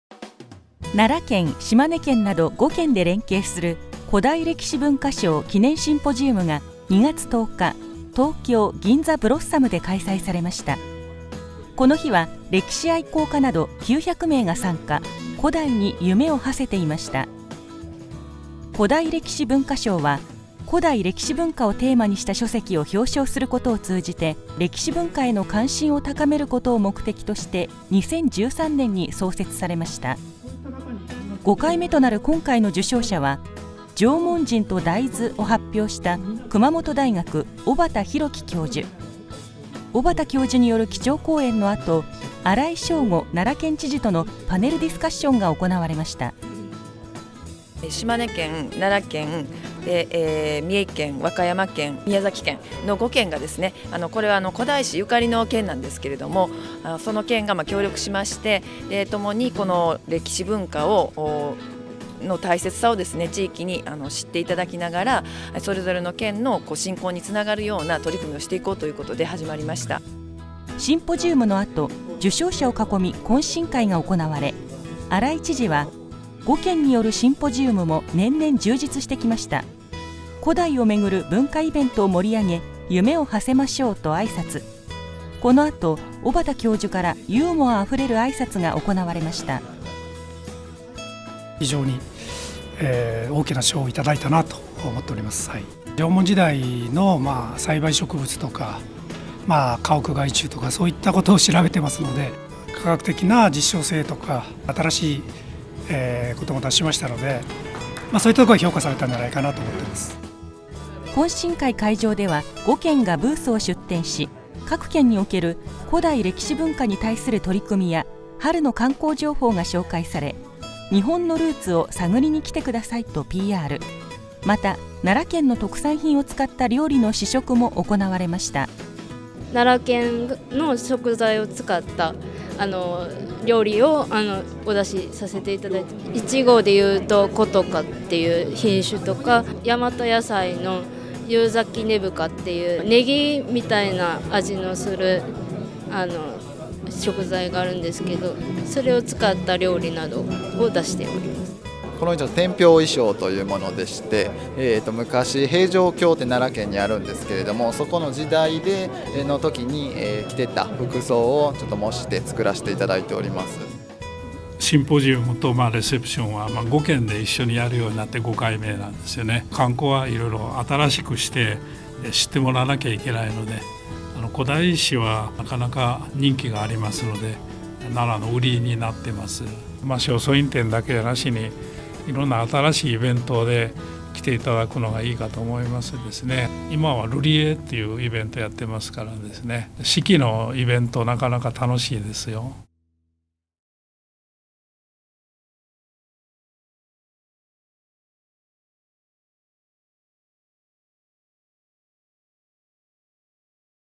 奈良県「第５回古代歴史文化賞 小畑教授 記念講演」
【フードボイス】奈良県、島根県など５県で連携する「古代歴史文化賞」記念シンポジウムが２月１０日、東京・銀座ブロッサムで開催された。この日は歴史愛好家など９００名が参加、古代に夢をはせていた。「古代歴史文化賞」は、古代歴史文化をテーマにした書籍を表彰することを通じて、多くの人の歴史文化への関心を高めることを目的として２０１３年に創設された。５回目となる今回の受賞者は、「縄文人とダイズ」を発表した熊本大学 小畑弘己教授。小畑教授による基調講演の後、荒井正吾 奈良県知事とのパネルディスカッションが行われた。